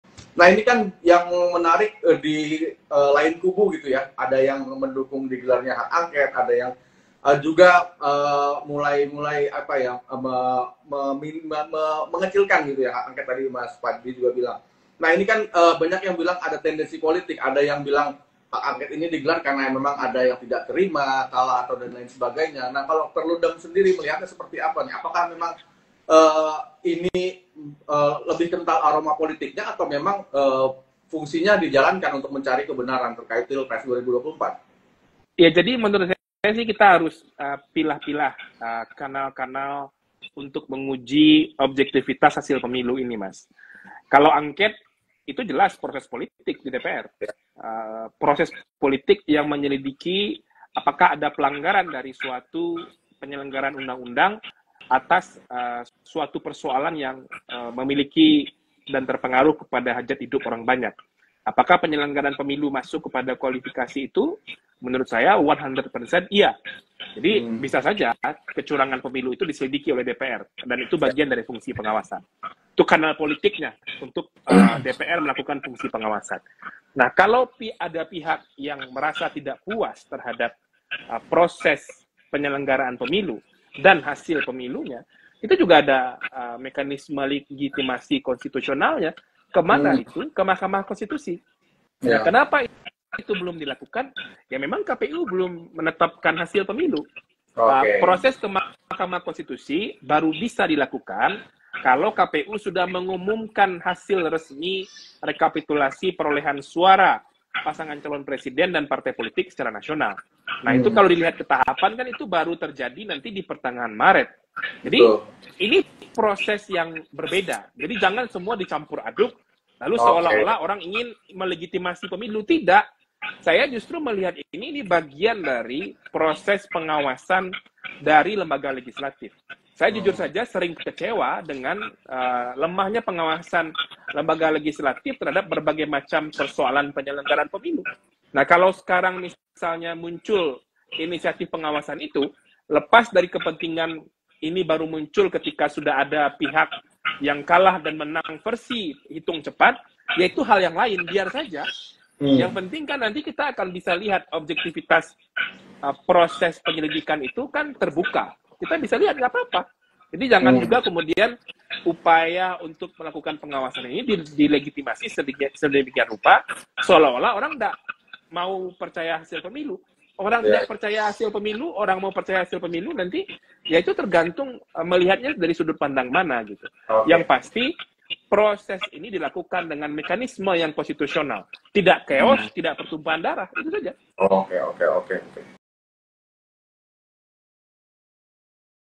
PERLUDEM MENILAI HAK ANGKET BAGIAN DARI PROSES PENGAWASAN
Perludem melihat wacana hak angket kecurangan pemilu bagian dari proses pengawasan dari lembaga legislatif. Hal itu disampaikan Peneliti Perludem, Fadli Ramdahanil.